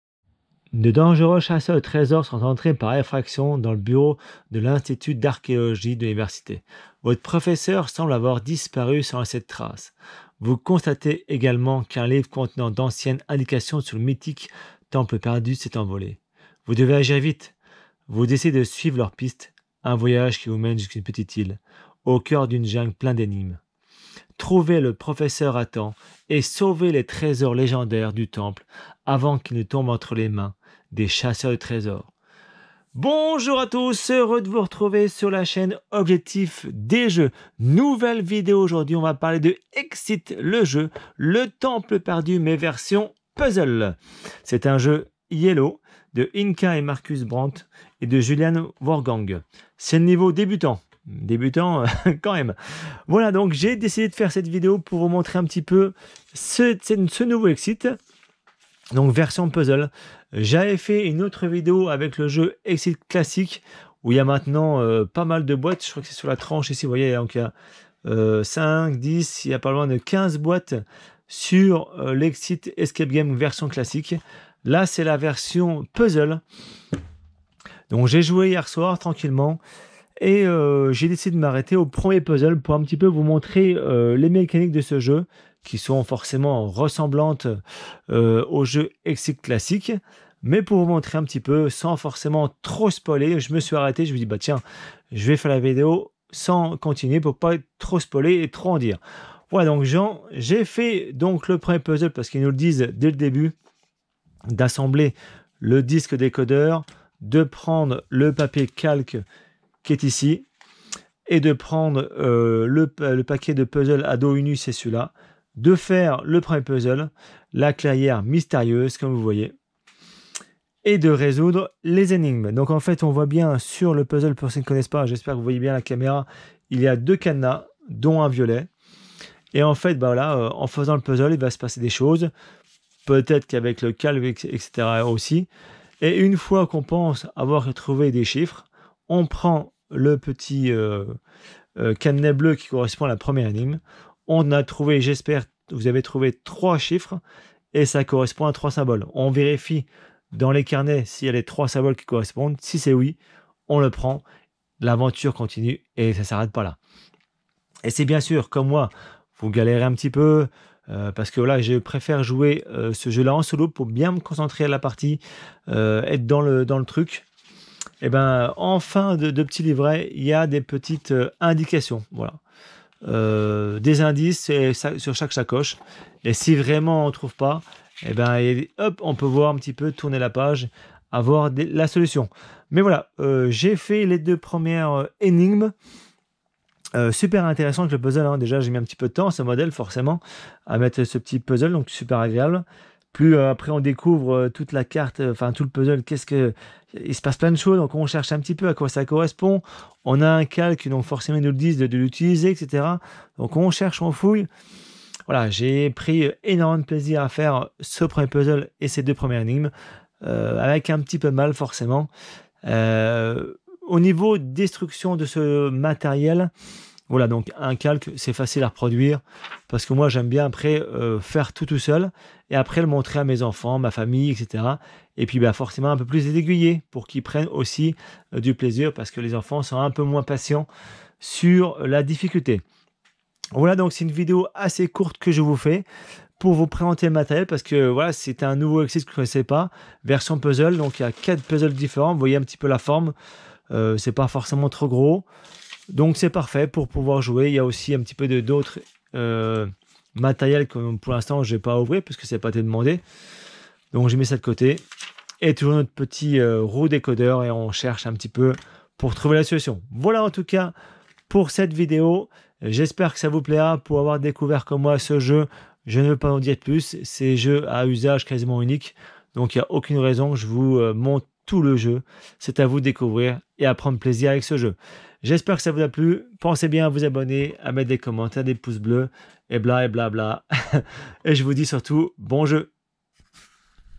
Exit Puzzle Le Temple Perdu règle du jeu en vidéo et en Français. IELLO. Inka & Markus Brand
jeu de société Exit Puzzle : Le Temple Perdu

Découverte du jeu de société "Exit Puzzle'' Le Temple Perdu chez l'éditeur Iello
Présentation, explication d'une partie et de ses règles en vidéo.
EXIT est un jeu qui reprend les sensations des "escape games". Muni d'indices, de matériel et d'un décodeur, vous aurez pour mission de sortir du jeu le plus rapidement possible.

Auteurs du jeu de société Exit Puzzle : Le Temple Perdu : Inka & Markus Brand, Juliane Voorgang

Si vous aimez cette chaîne @objectifdésjeux
Si vous souhaitez nous montrer votre soutien :
Un like - Un commentaire - un partage
A bientôt 👋